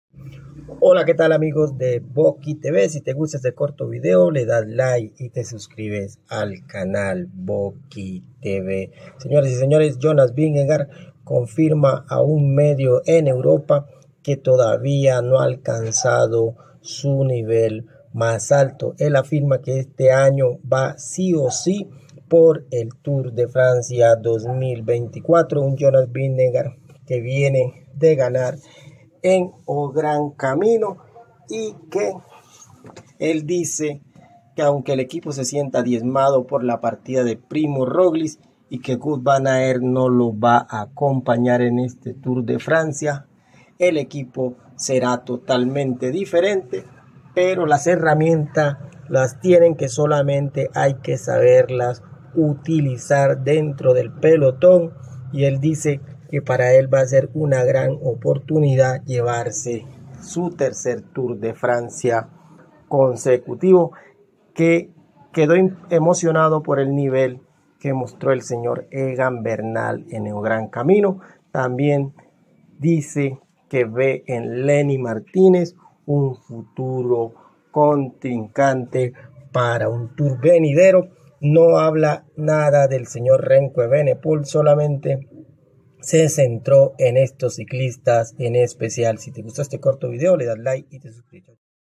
Aun no alcanzo mi maximo nivel jonas vingegaar
El danez jonas vingegaar sigue con su preparacion para el tour 2024